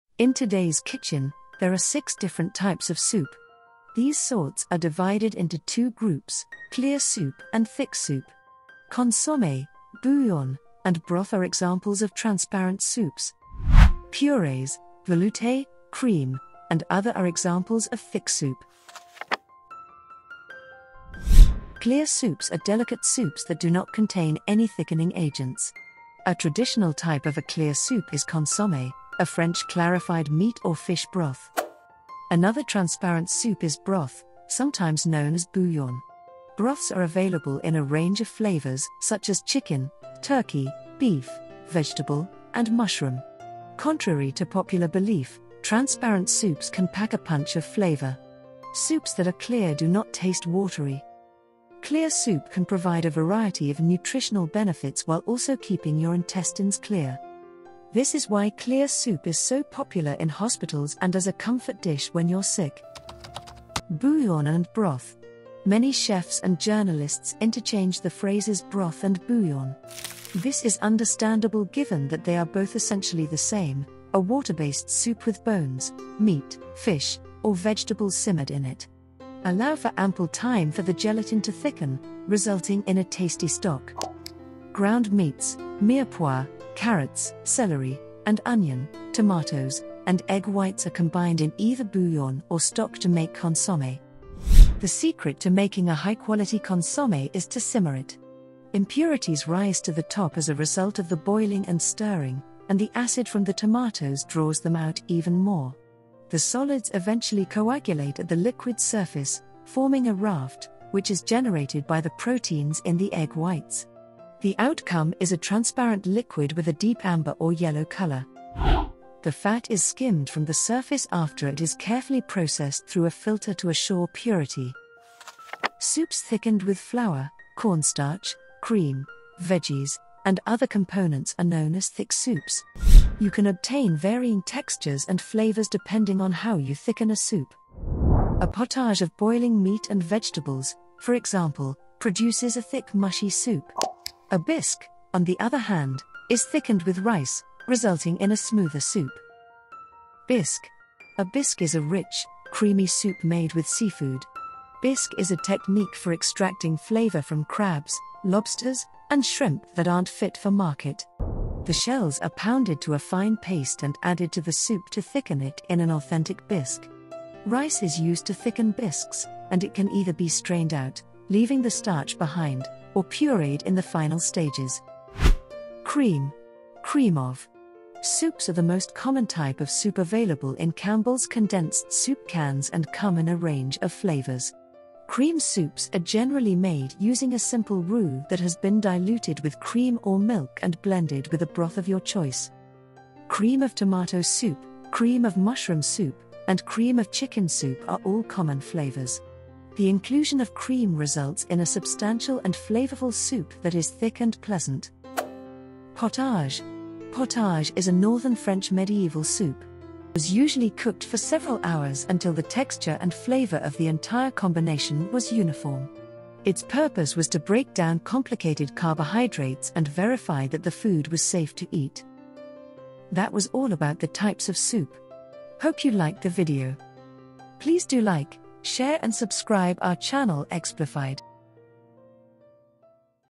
The 6 Types Of Soup (Beware: Watching This Video Might Make You Hungry)
In today's kitchen, there are six different types of soup. These sorts are divided into two groups: clear soup and thick soup. Consommé, bouillon, and broth are examples of transparent soups. Purees, velouté, cream, and other thick soups are examples. Watch this video to learn about the types of soup.